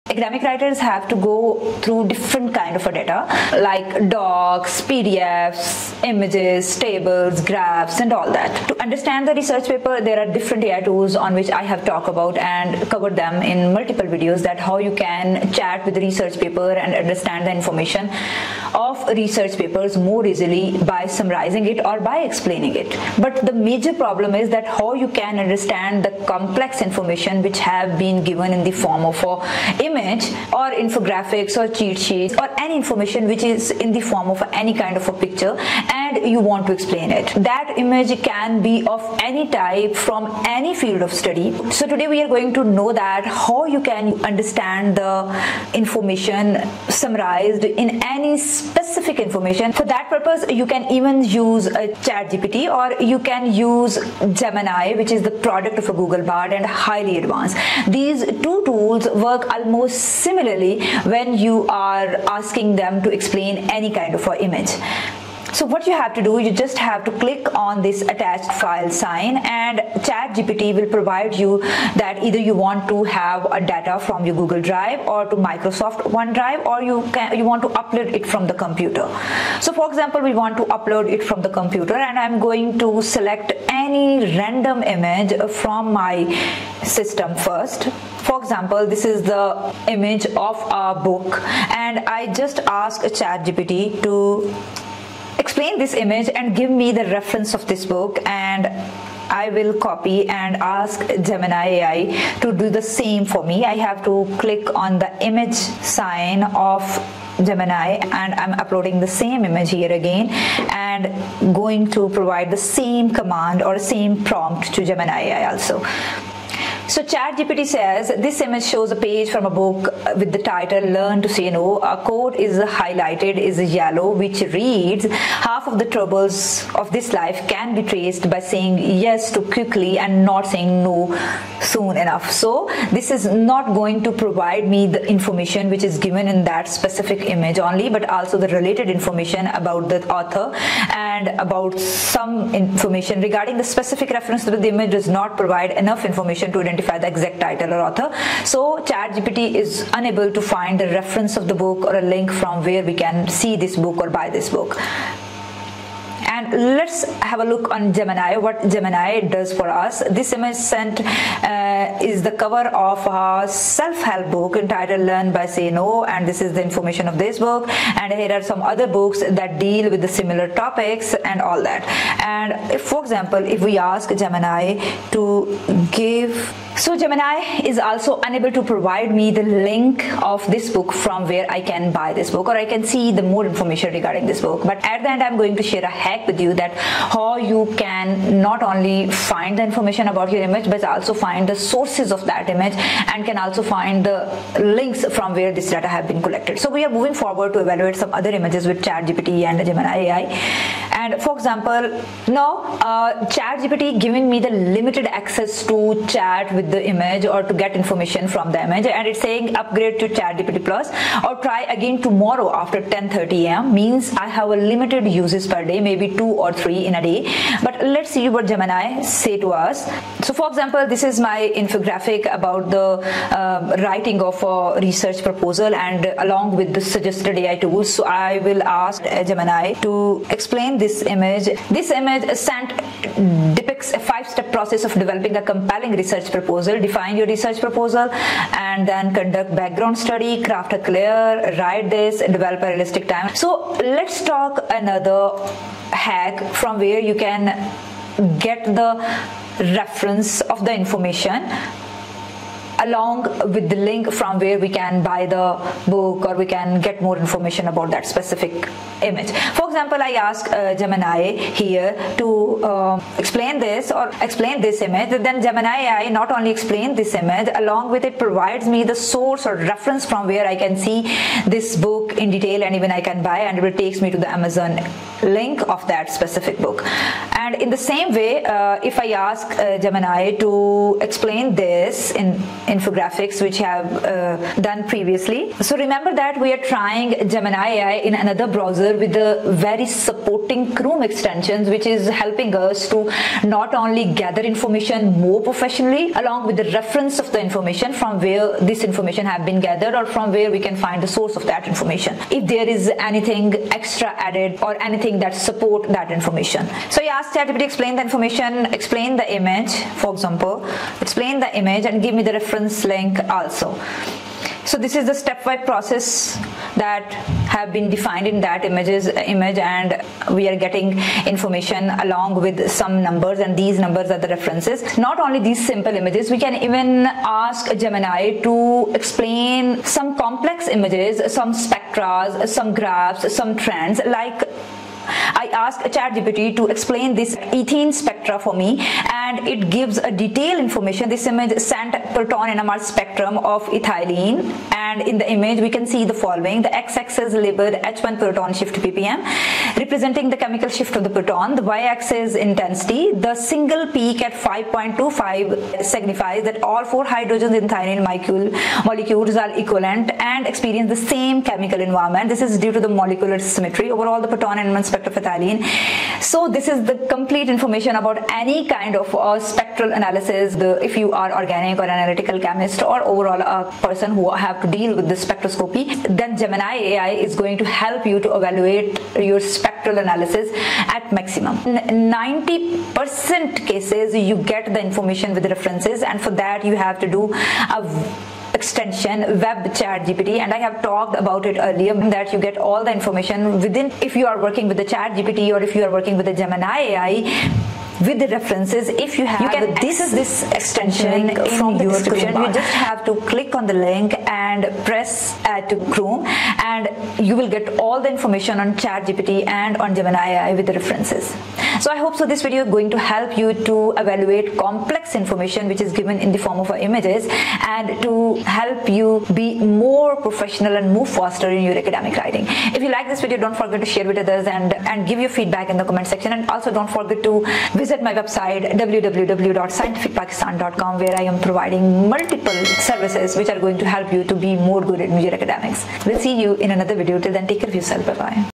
Read Complex Images & Diagrams With FREE AI Tools
Read complex scientific images and structures with Free AI Tools
Use Gemini ai in free